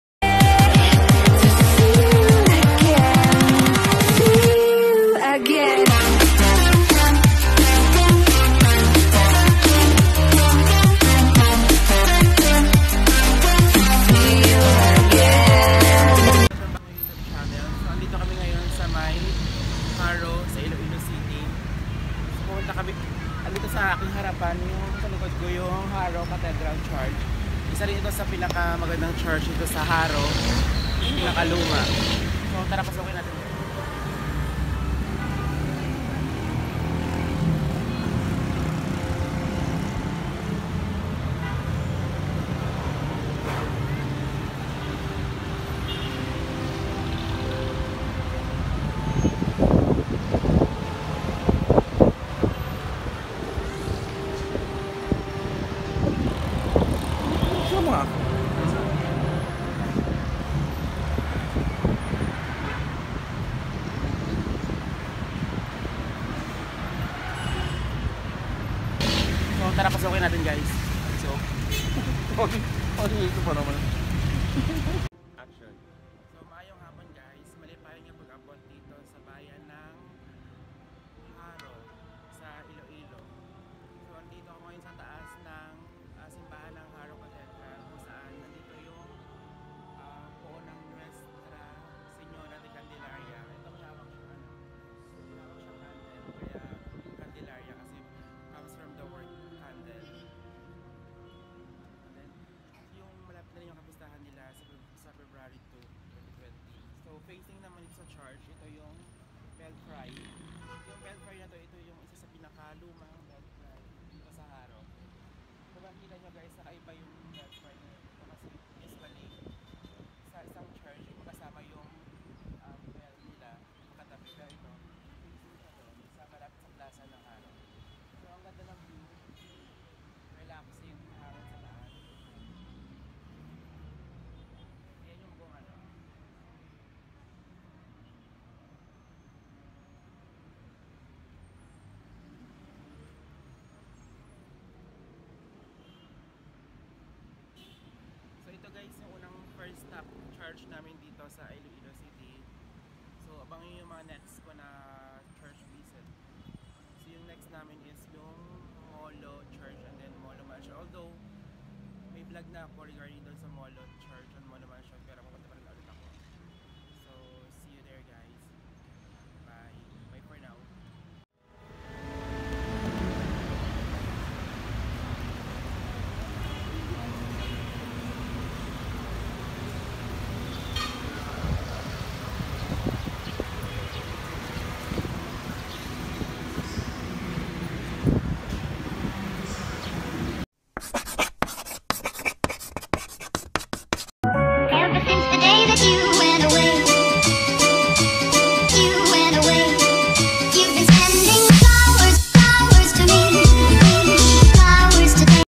Vlog#23 Jaro Cathedral and the Belfry
The Jaro Cathedral was once destroyed by a quake in January 1948 and was only restored in 1956 under the order of the first archbishop of Jaro, His Excellency Jose Ma. Cuenco.

The Jaro belfry stands apart from the main church and can be seen just in front of the cathedral. The Jaro Cathedral is the first and only cathedral in the province.